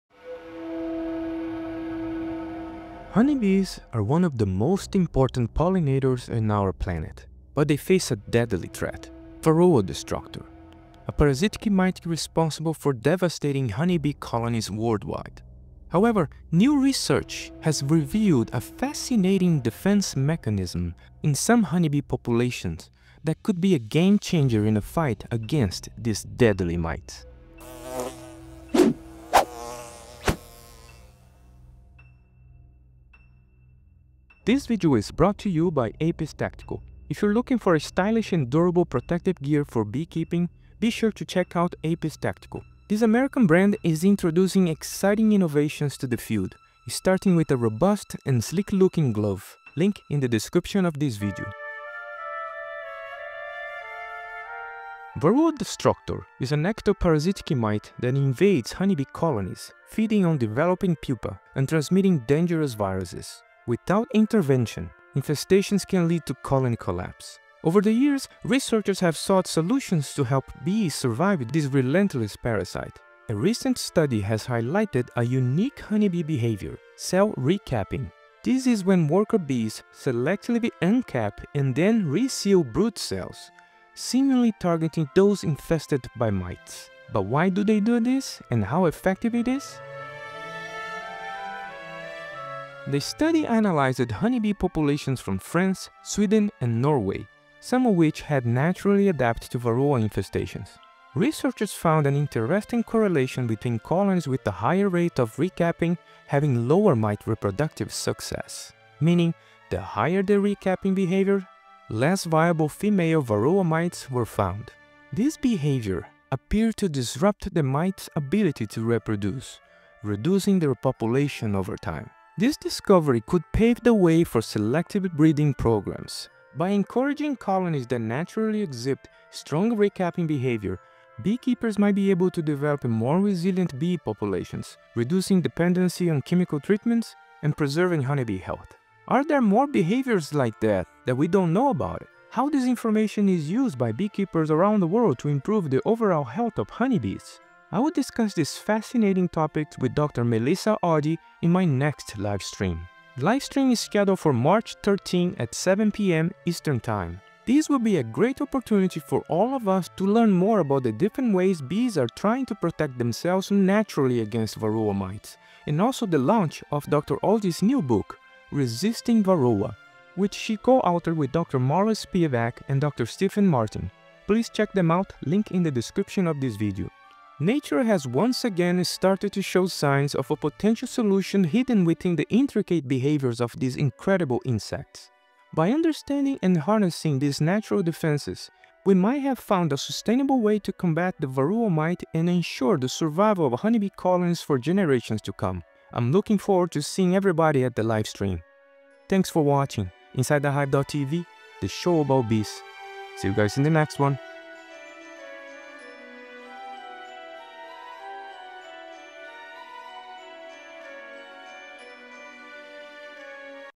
New HONEY BEE BEHAVIOUR to fight VARROA MITES.
In this episode I cover some interesting results Dr. Melissa Oddie found, studying honey bees that naturally resist varroa mites in nature.
Reach out on social media.